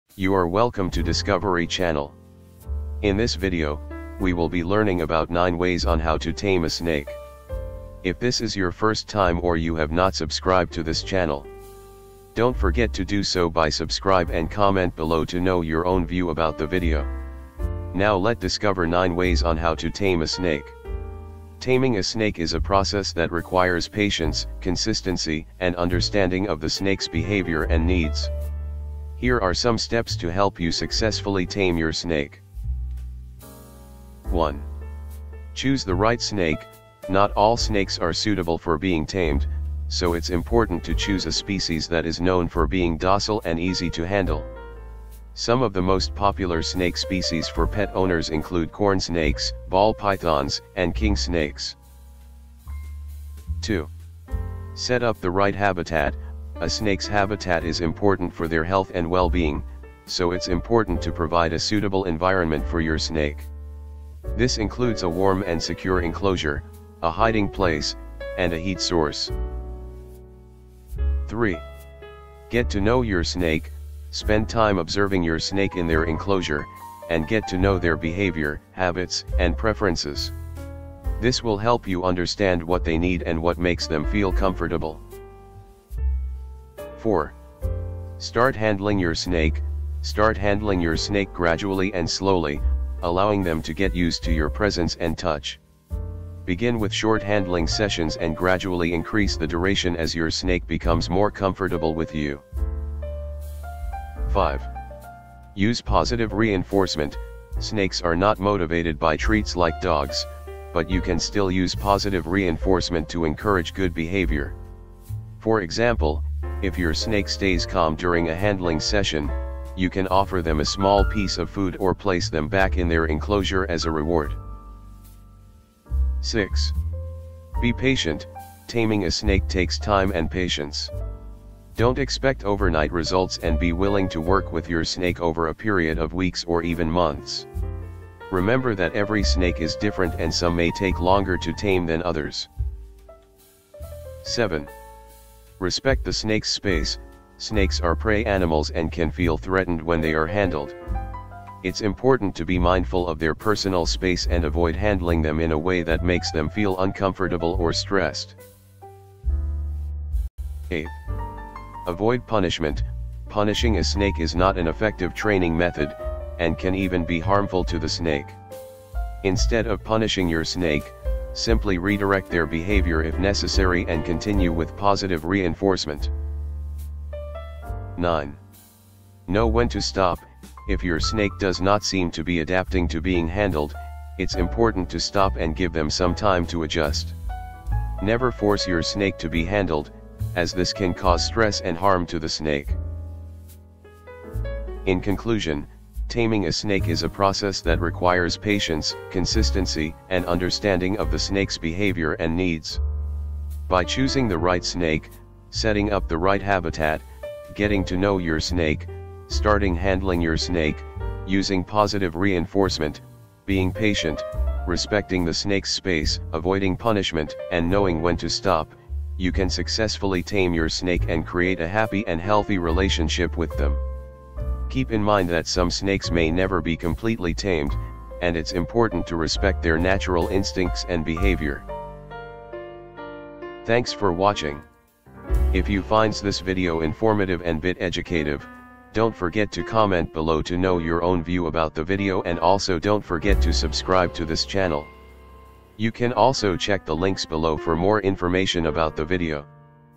9 Ways On How To TAME SNAKE - Snakes Discovery Channel (Documentary)
9 Ways On How To TAME SNAKE - Snakes Discovery Channel (Documentary)

In this video, we're going to take a look at some of the most dangerous snakes in the world. From the deadly black mamba to the deadly coral snake, these snakes are capable of inflicting serious damage.

WARNING: This video is not for the faint of heart! If you're afraid of snakes, then you might want to skip this one. But if you're curious about what makes these snakes so dangerous, then buckle in – we're going to take a look!